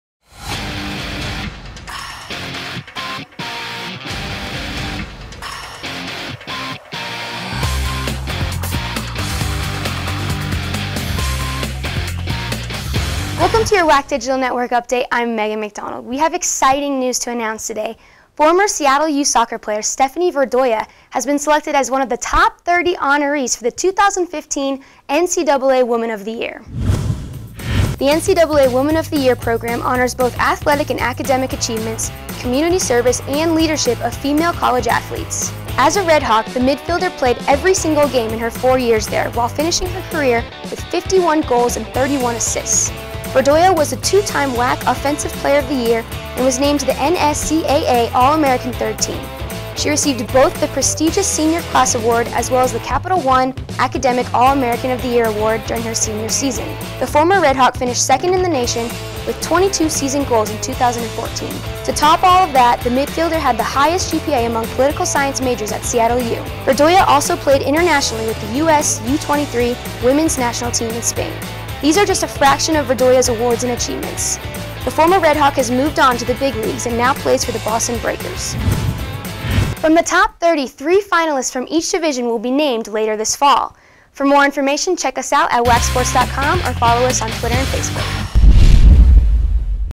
Stephanie Verdoia 2015 NCAA Woman of the Year Nominee
Seattle U women's soccer player Stephanie Verdoia has been selected as one of the top 30 honorees for the 2015 NCAA Woman of the Year. The award honors athletic and academic achievements, community service and leadership of female college athletes.